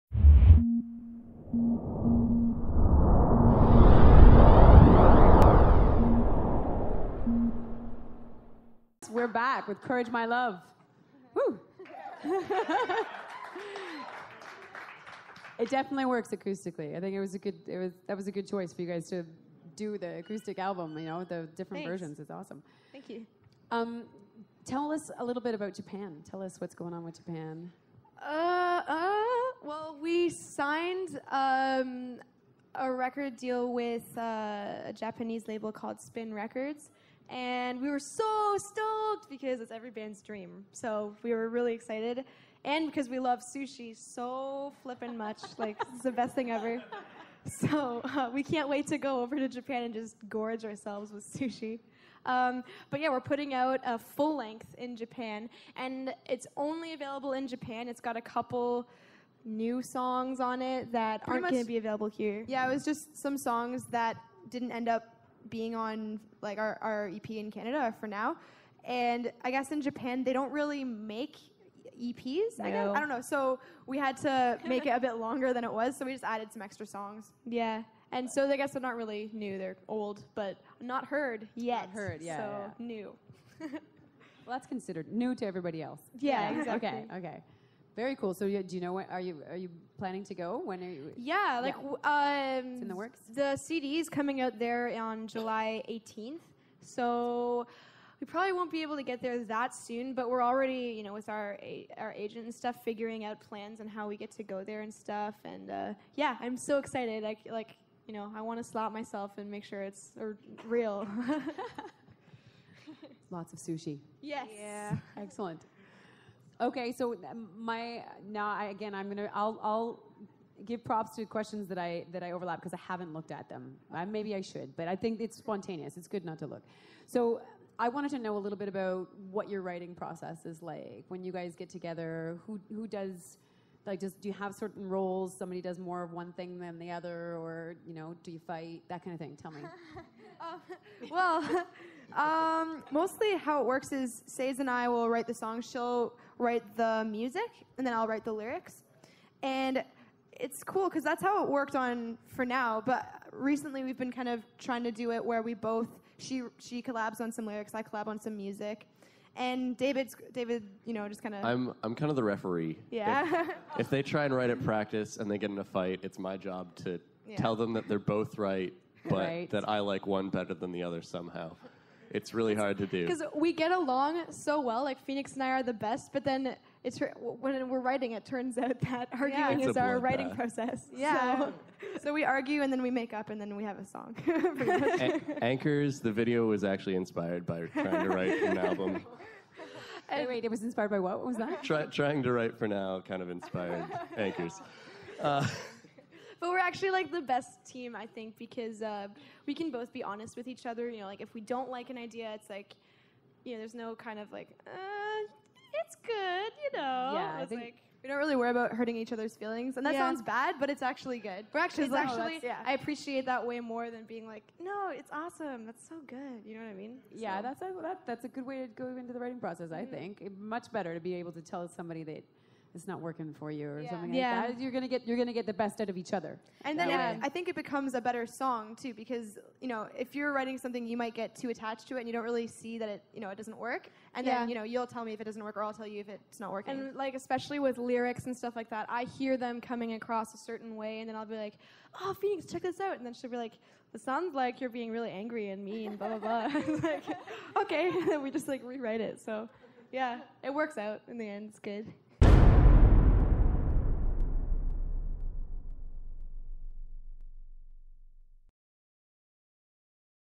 Courage My Love - Japan & Band Roles
Courage My Love doing a Backstage Pass interview and performance at Metalworks Institute.